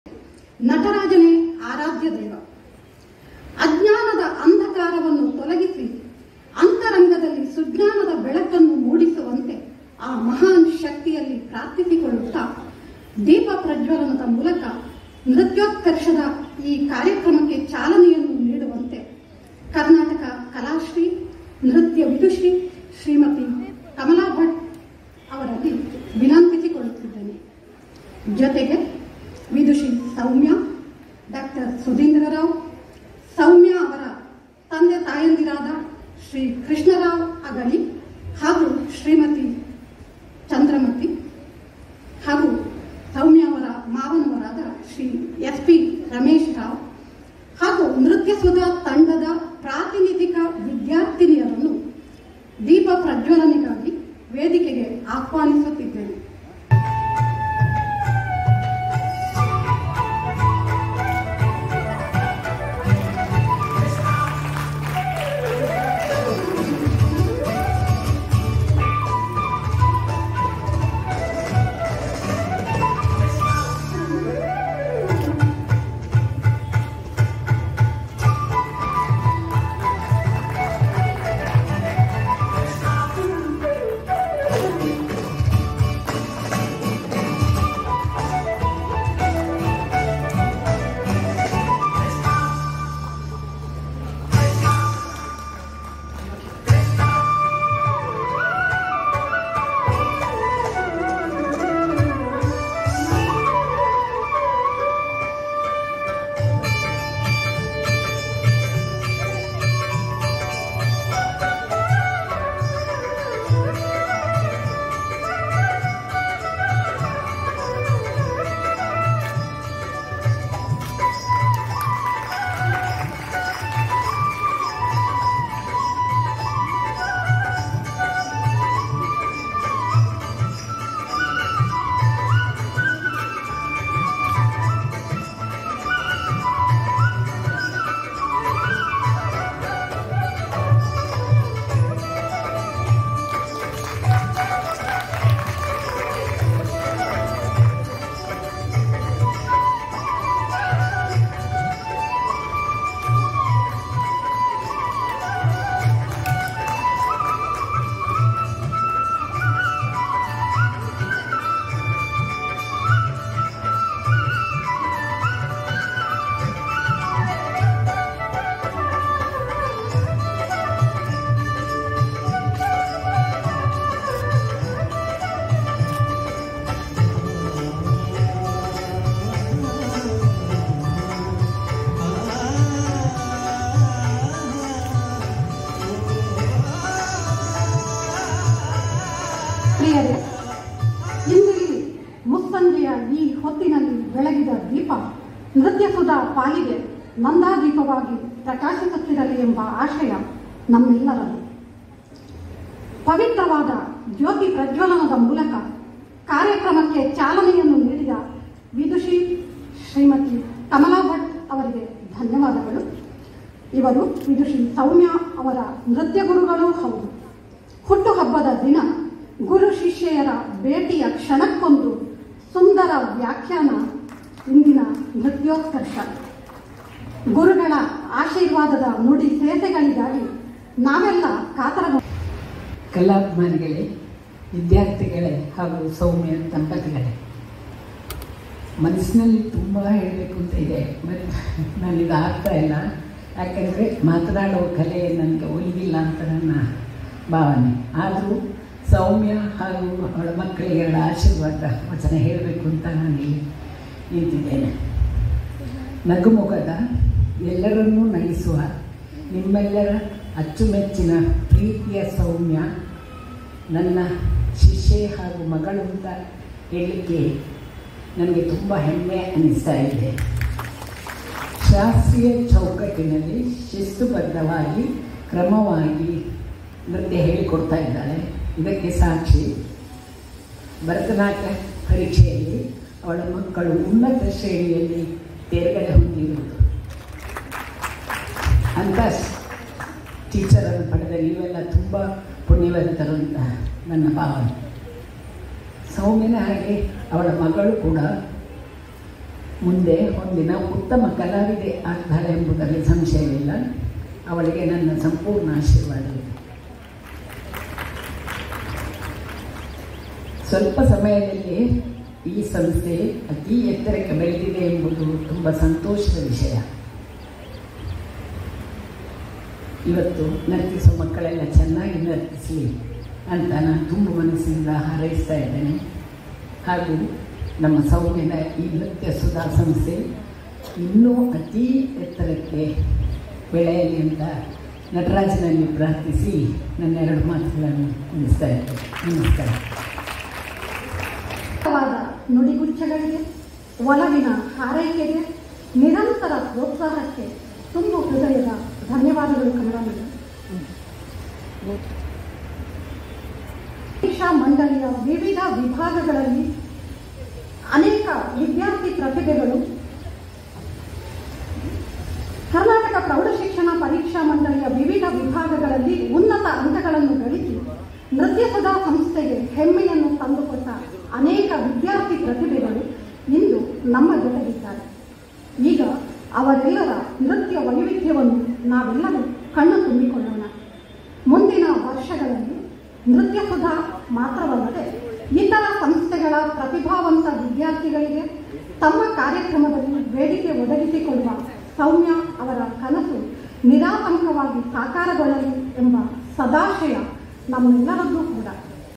NRITHYOTKARSHA -BY NRITHYASUDHA {EPISODE-1}SSR10
NRITHYOTKARSHA -DISCIPLES OF SOUMYA SUDHINDRA RAO PERFORMED RECENTLY AT TOWNHALL MANGALORE.